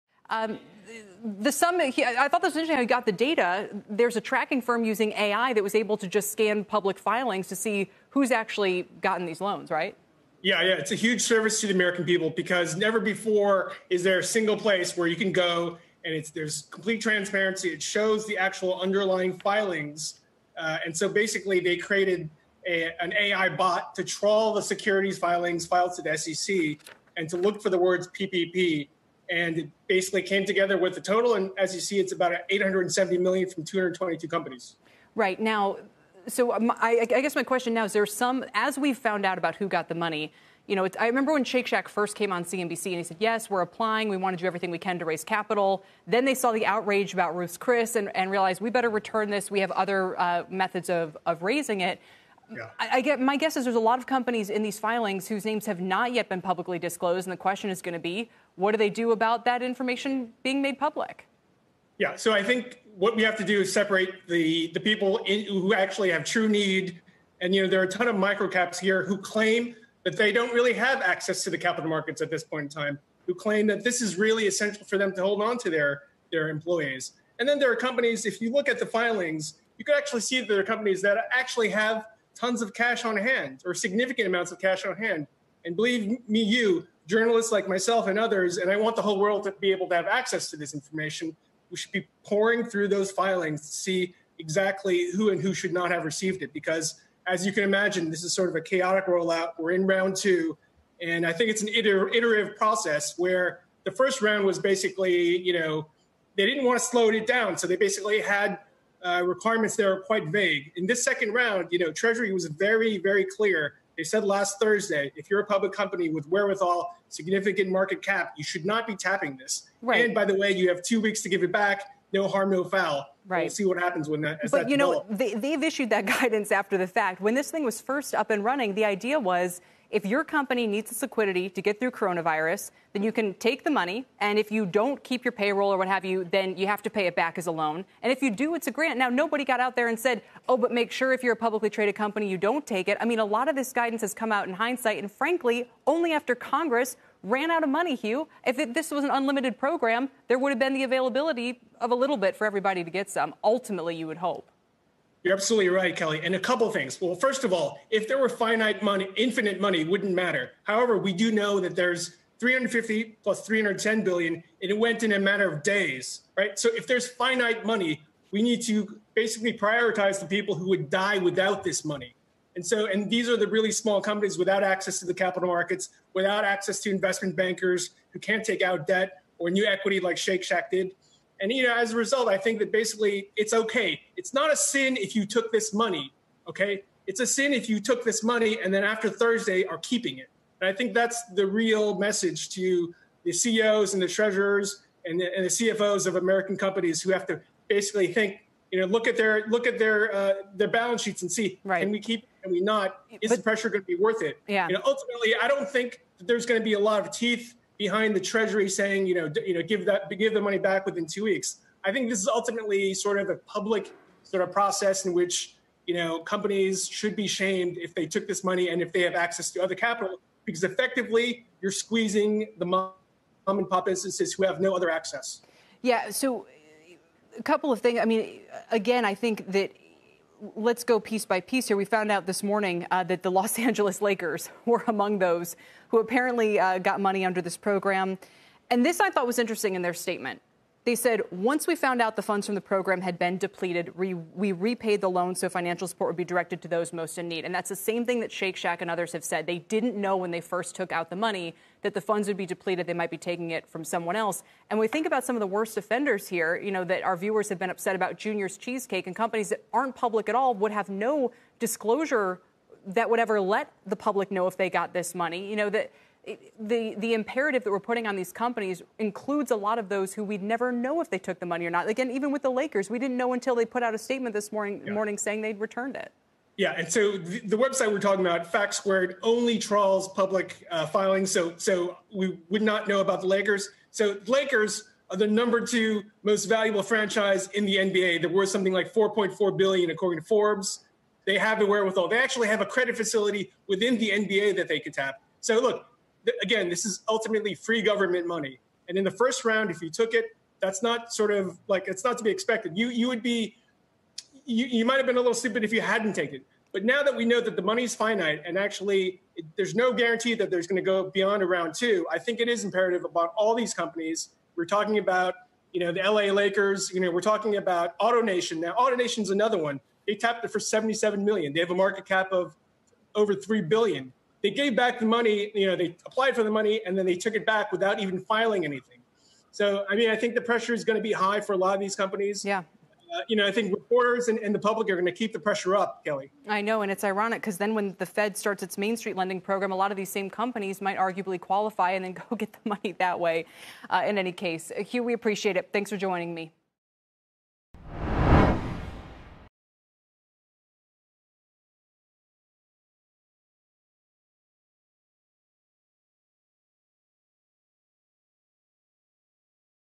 Over 220 companies applied for $870 million in PPP loans
More than 220 public companies applied for at least $870 million from the government program that was billed as emergency funding for small businesses without access to other sources of capital, according to data analytics firm FactSquared. CNBC's Hugh Son reports.

Even as the U.S. small business relief program is set to reopen Monday with fresh funding, the full extent that public companies tapped the emergency facility is only now becoming clear.

More than 220 public companies applied for at least $870 million from the government program that was billed as for small businesses without access to other sources of capital, according to Washington D.C.-based data analytics firm FactSquared.

The government’s Paycheck Protection Program sparked outrage after its initial $350 billion allotment quickly ran out and it was revealed that big public companies secured loans while hundreds of thousands of small businesses seeking relatively tiny amounts were left in limbo.

Last week, the Small Business Administration attempted to close that loophole, saying that big public companies “with substantial market value and access to capital markets” aren’t eligible and that firms that already tapped the fund had two weeks to return the PPP money.

Since then, companies including Ruth’s Hospitality Group and sandwich chain Potbelly have followed Shack Shack in returning their PPP funds.

CNBC TV